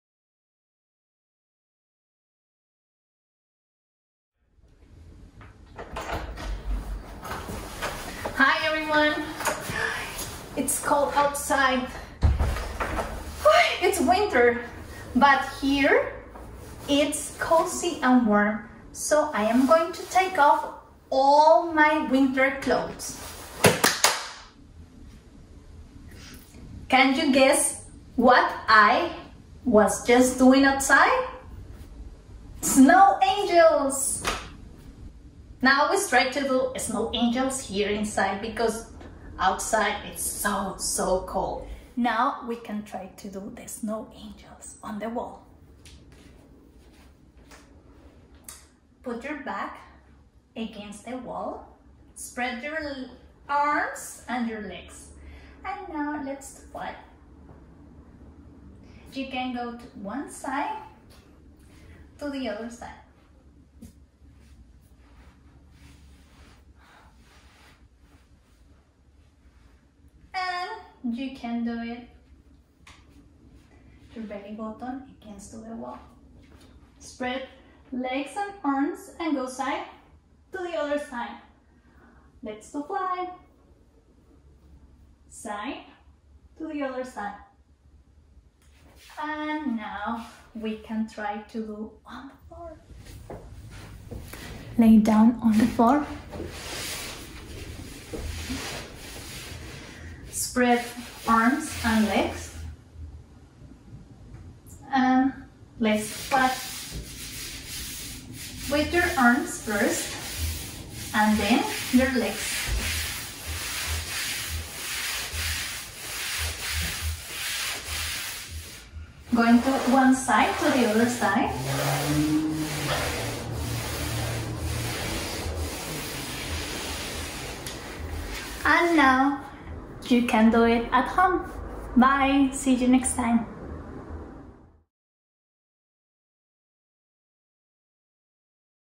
Project Commotion - Creative Movement Class Winter - Carol (ASL)
Practice snow angels at home with Carol

ASL Interpreter: Deb'e Taylor